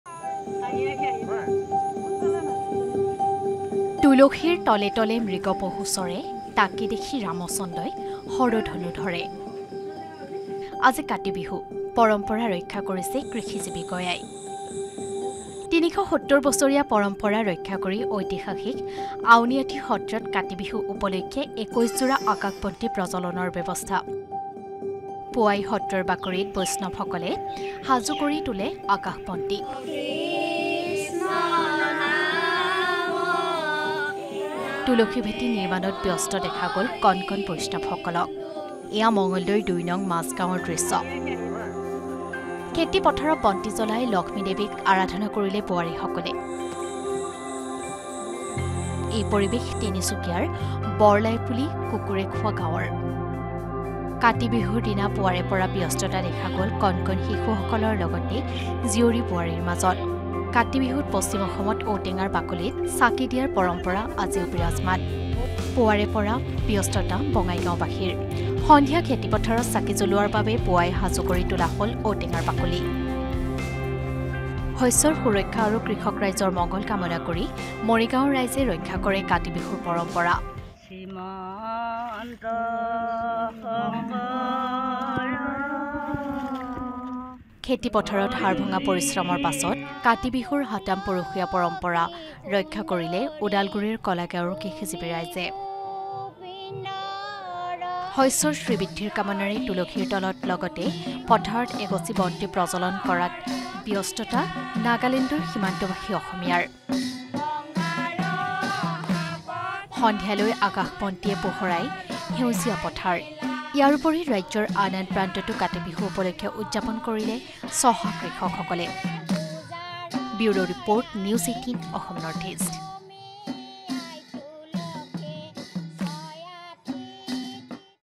Kati Bihu 2023 : ৩৭০ বছৰীয়া পৰম্পৰা ৰক্ষা কৰি আউনিআটী সত্ৰত ২১যোৰা আকাশবন্তী প্ৰজ্বলনৰ ব্যৱস্থা N18V
Kati Bihu 2023 :  তুলসীৰ তলে তলে মৃগ পহু চৰে, তাকে দেখি ৰামচন্দ্ৰই হৰধনু ধৰে। আজি কাতি বিহু। তুলসী তলৰ চাকিগছি আৰু আকাশবন্তিৰে উদ্ভাসিত হ’ব অসমৰ ইমূৰৰ পৰা সিমূৰ। ৰাজ্যজুৰি চলিছে কাতি বিহু পালনৰ প্ৰস্তুতি